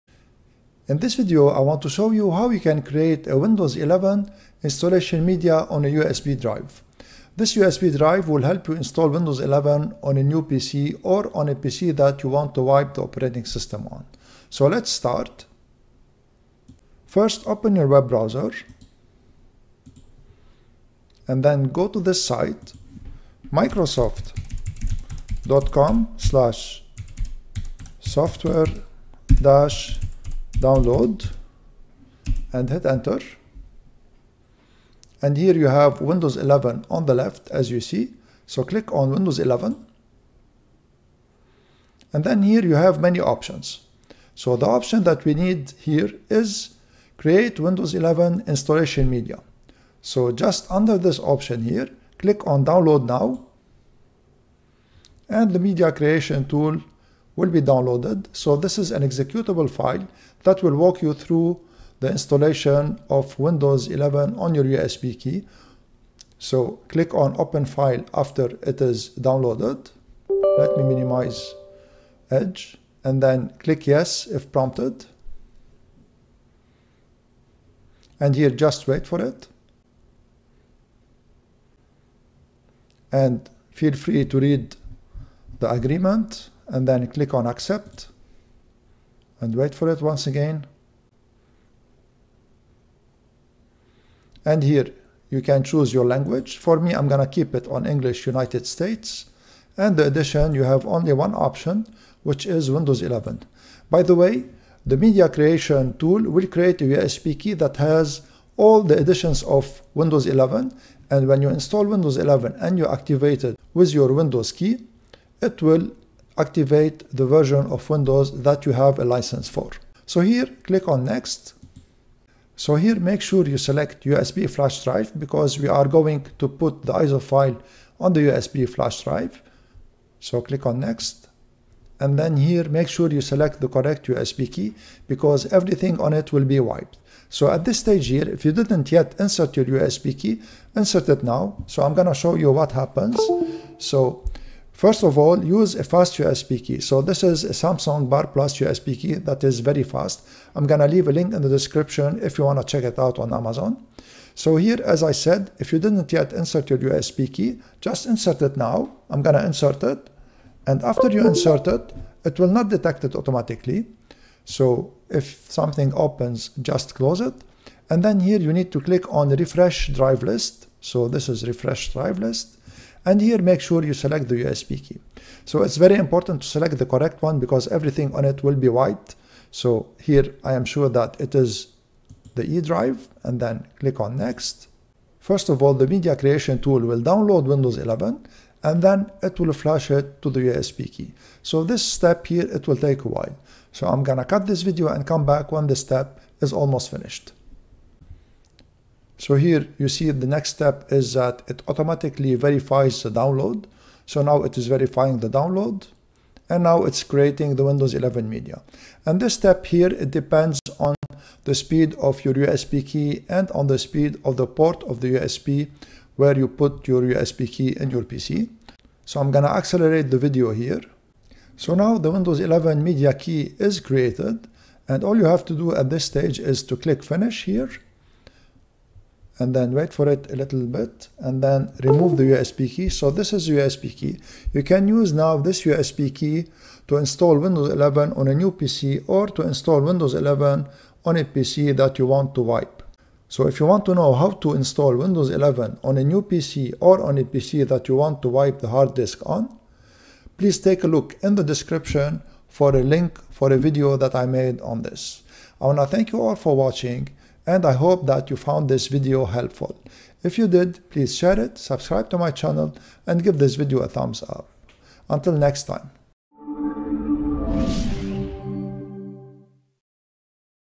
How to create Windows 11 USB installation media
Create Official Windows 11 USB key installation media with Microsoft media Creation tool
Link to the Samsung Bar Plus USB key

Follow me on:
My websites: